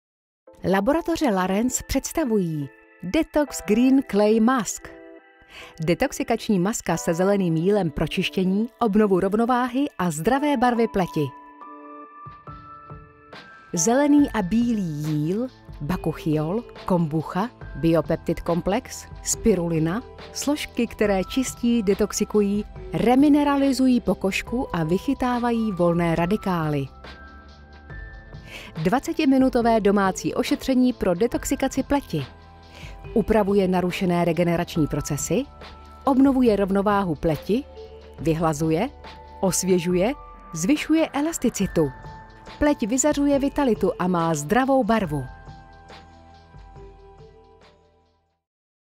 CZ Detox Green Clay Mask
Díky kombinaci složek s intenzivním a hloubkovým čistícím a detoxikačním účinkem účinně detoxikuje pokožku, a tím obnovuje její sílu a rovnováhu. Pokud chcete, aby Vaše pleť vyzařovala vitalitu a měla zdravou barvu, určitě se spřátelte s tímto opojně vonícím zeleným zázrakem💚